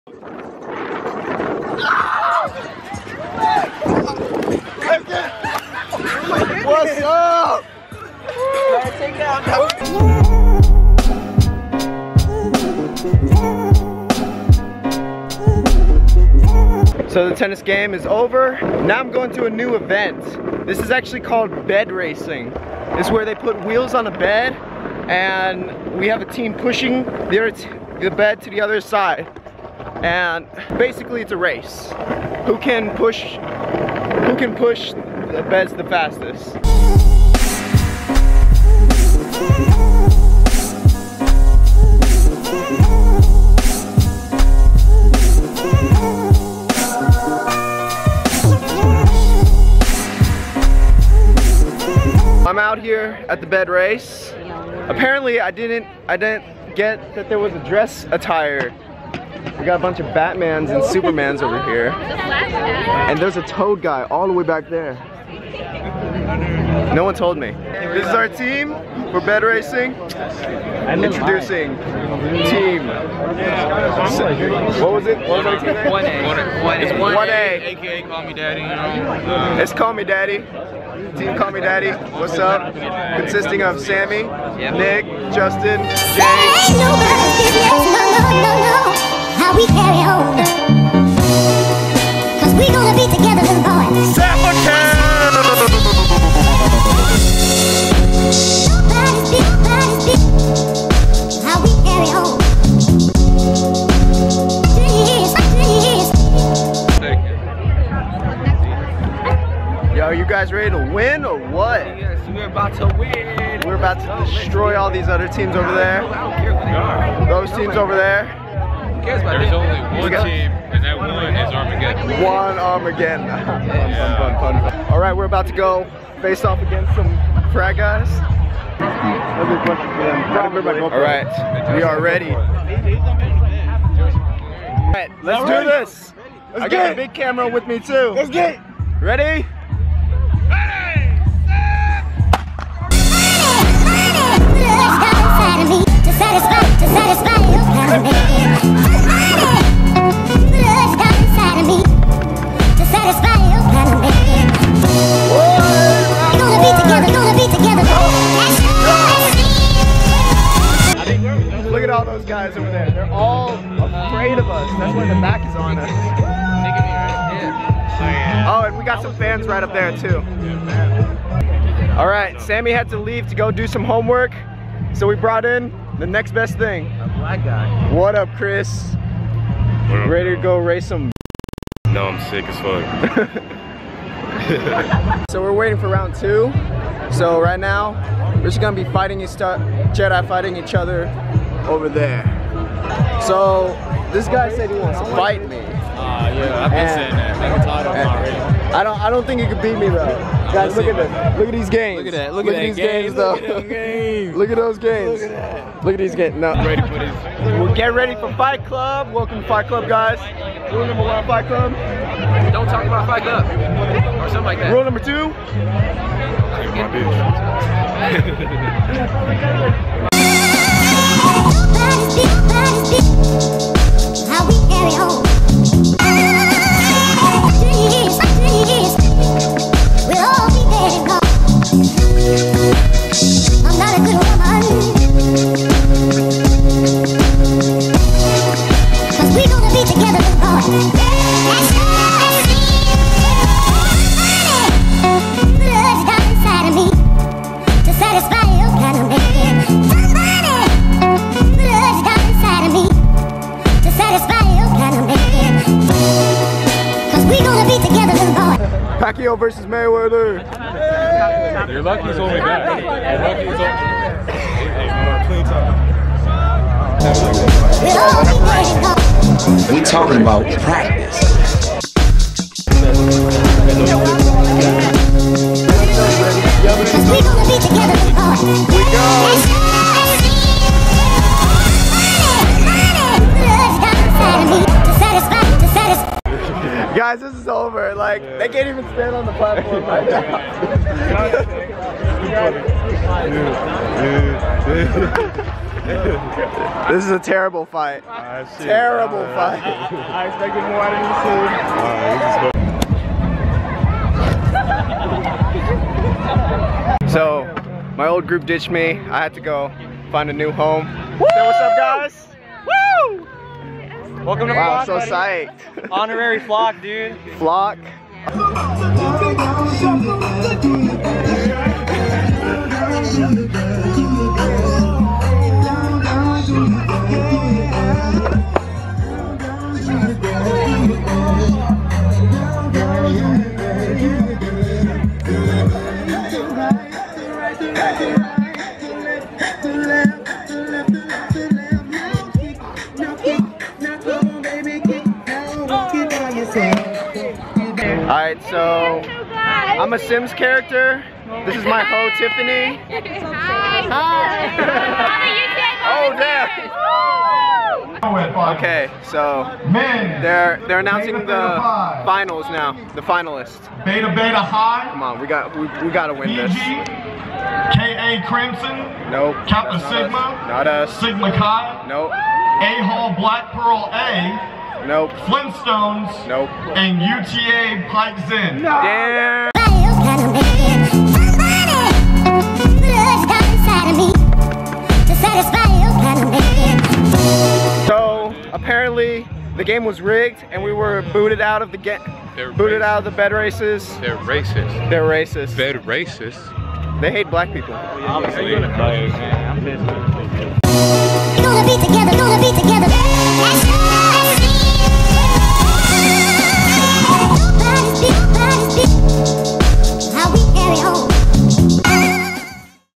Bed Racing Frats and Fightig On Floats
Just an even held on campus with a couple turnt dudes.

Follow me to keep up!